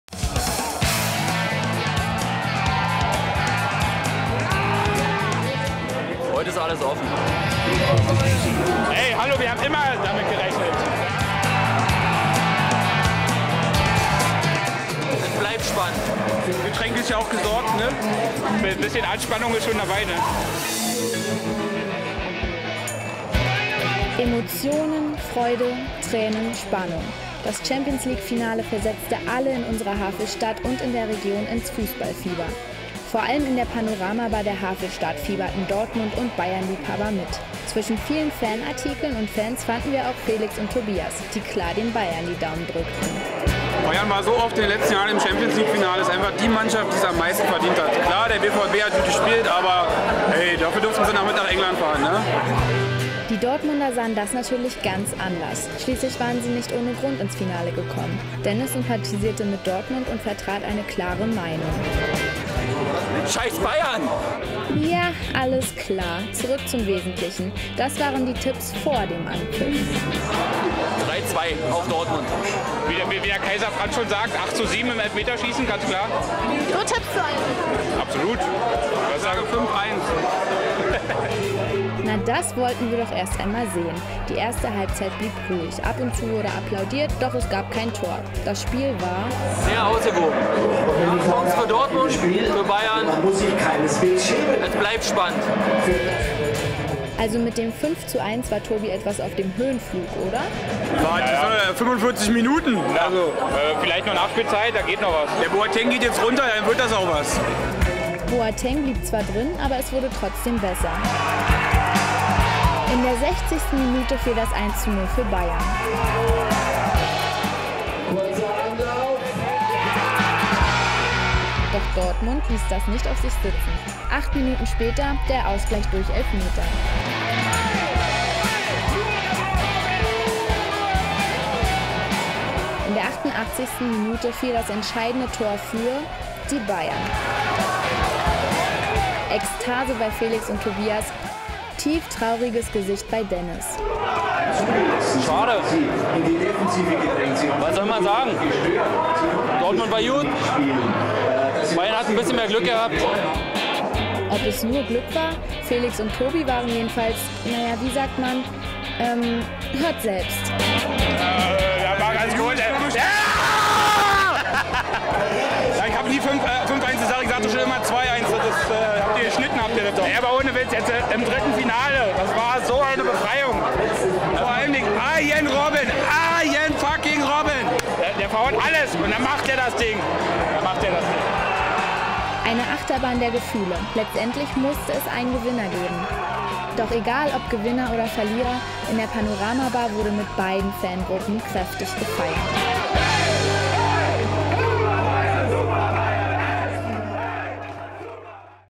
SKB HD | Champions-League-Finale in der Havelstadt
Was für eine Party. In Brandenburg an der Havel fieberten die Fans der Bayern und Dortmunder bis zur letzten Minute - der dann folgende Jubel "der Roten" war unbeschreiblich. Versöhnlich: Nach dem Triumph feierten beide Lager zusammen bis in die Nacht.